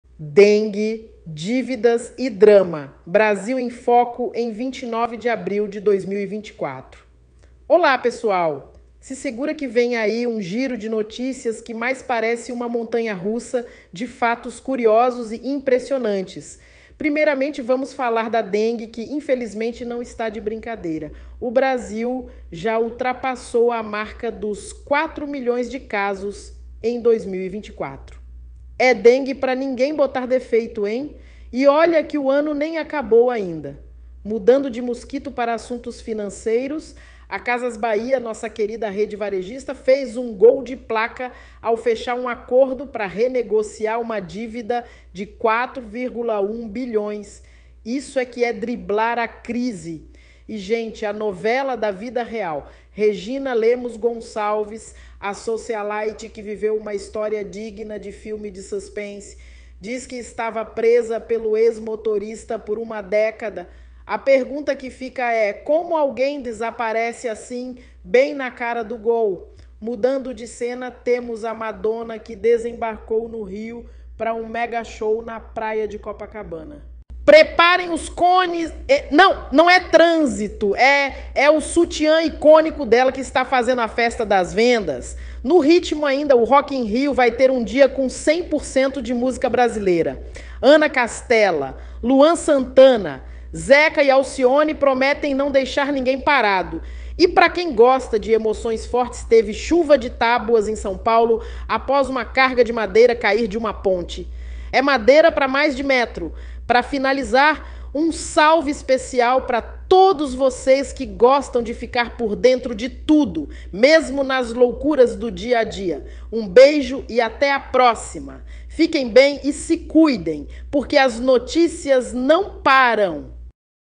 Dengue, Dívidas e Drama: Brasil em Foco em 29 de Abril de 2024
Olá, pessoal! Se segura que vem aí um giro de notícias que mais parece uma montanha-russa de fatos curiosos e impressionantes. 🎢

Primeiramente, vamos falar da dengue que, infelizmente, não está de brincadeira: o Brasil já ultrapassou a marca dos 4 milhões de casos em 2024. É dengue pra ninguém botar defeito, hein? E olha que o ano nem acabou ainda!

Mudando de mosquito para assuntos financeiros, a Casas Bahia, nossa querida rede varejista, fez um gol de placa ao fechar um acordo para renegociar uma dívida de R$ 4,1 bilhões. Isso é que é driblar a crise!

E, gente, a novela da vida real: Regina Lemos Gonçalves, a socialite que viveu uma história digna de filme de suspense. Diz que estava presa pelo ex-motorista por uma década! A pergunta que fica é: como alguém desaparece assim, bem na cara do Gol?

Mudando de cena, temos a Madonna que desembarcou no Rio para um megashow na Praia de Copacabana. Preparem os cones... não, não é trânsito, é o sutiã icônico dela que está fazendo a festa das vendas!

No ritmo ainda, o Rock in Rio vai ter um dia com 100% de música brasileira. Ana Castela, Luan Santana, Zeca e Alcione prometem não deixar ninguém parado.

E para quem gosta de emoções fortes, teve 'chuva' de tábuas em São Paulo após uma carga de madeira cair de uma ponte. É madeira pra mais de metro!

Para finalizar, um salve especial para todos vocês que gostam de ficar por dentro de tudo, mesmo nas loucuras do dia a dia. Um beijo e até a próxima! 💋 Fiquem bem e se cuidem, porque as notícias não param!